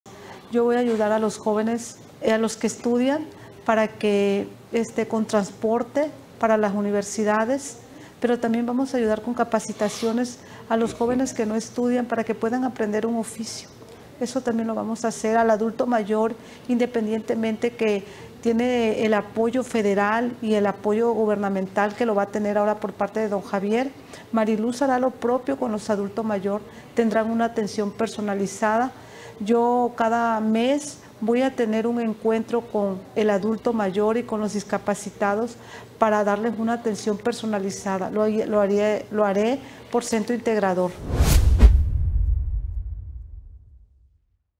Mari Luz Velázquez , otorgará transporte universitario y capacitaciones laborales para jóvenes.
👥EntrevistaDeAhora| Mari Luz Velázquez Jiménez, alcaldesa electa de Huimanguillo, nos comentó que ayudará a los jóvenes estudiantes con transportes para las universidades. De igual manera implementará capacitaciones para los jóvenes que no estudian y así puedan aprender un oficio.

Por su parte, Mari Luz ayudará a los adultos mayores, para que tengan una atención personalizada realizando encuentros mensuales por centro integrador.